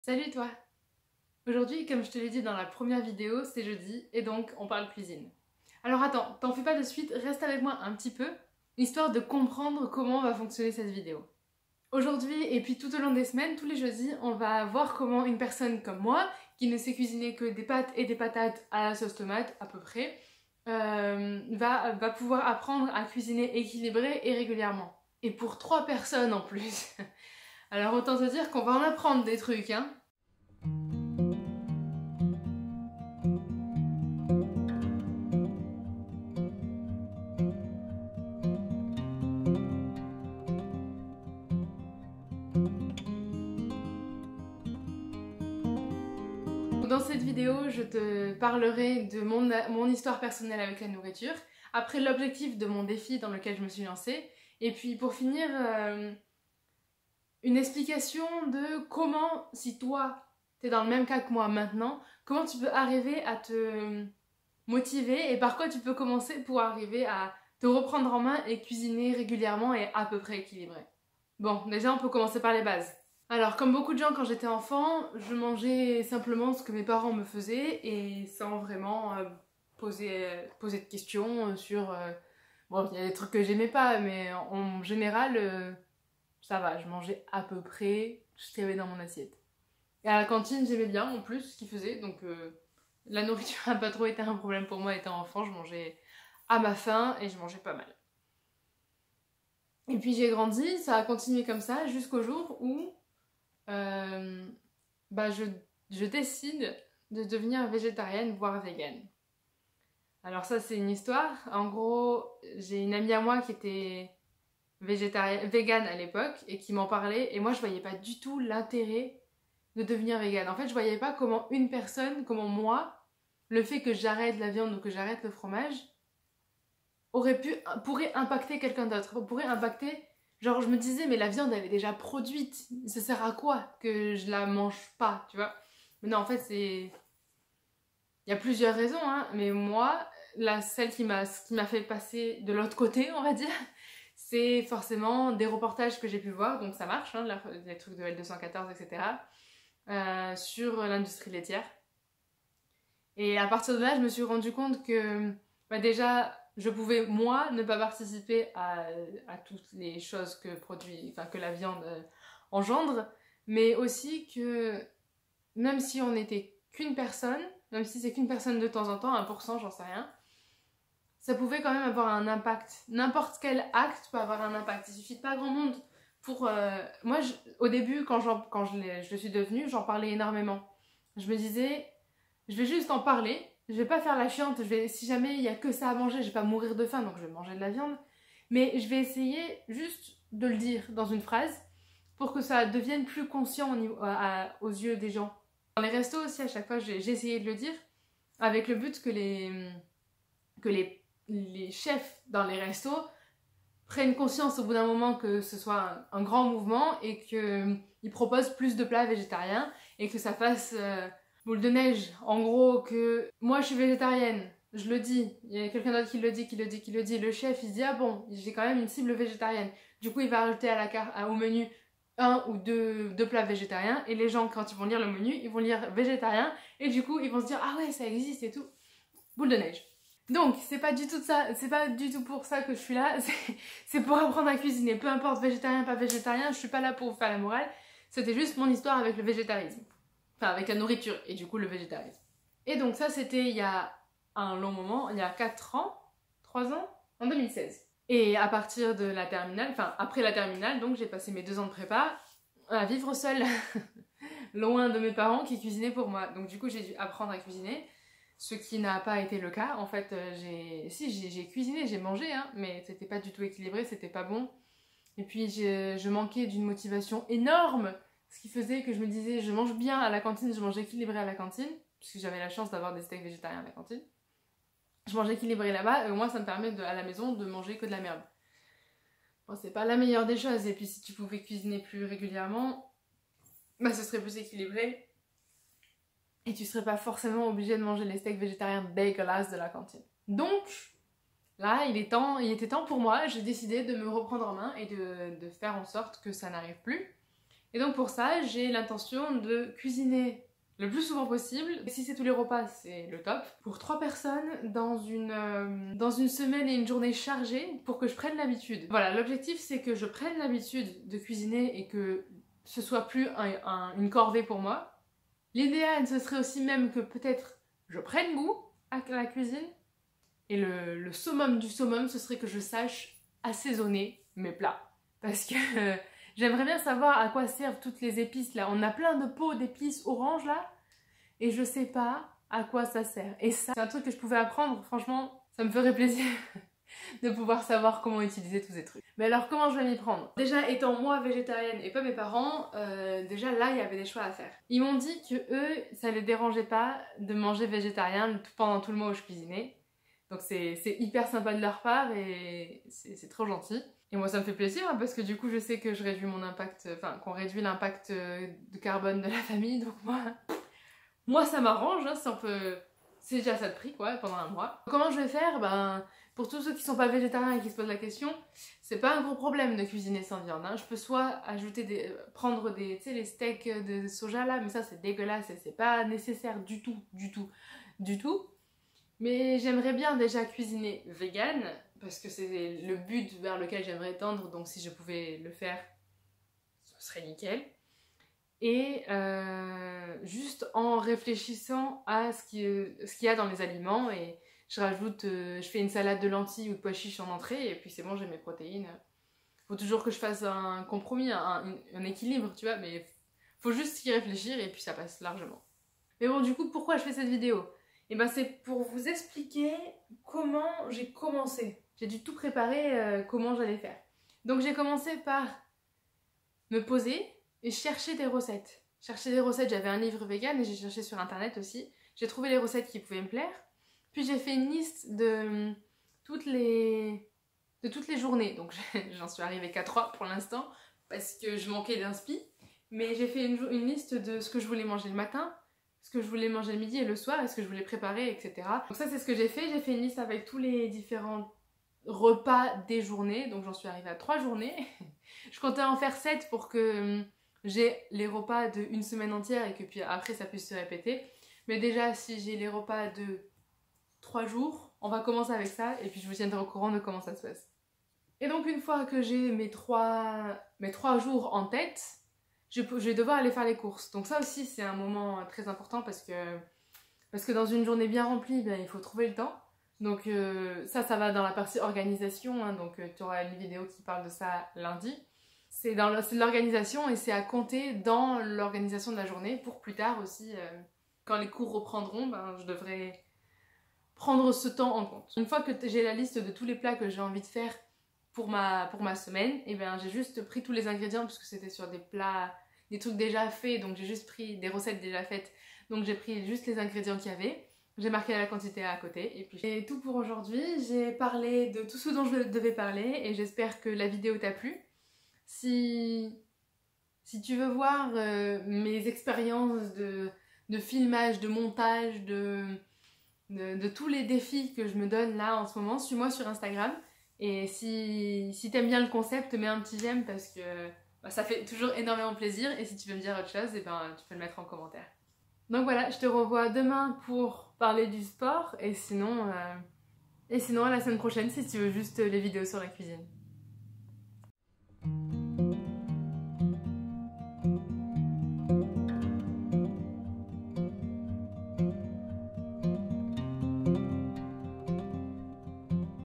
Comment faire à manger ? - Partage d'expérience - Jour 3
Dans cette vidéo je te montre comment je me suis lancée dans le défi de cuisiner tous les jours, équilibré et pour 3 personnes.

Je n'ai pas encore eu le temps de te mettre toutes les recettes ici, mais elles vont arriver ! (normalement assez vite)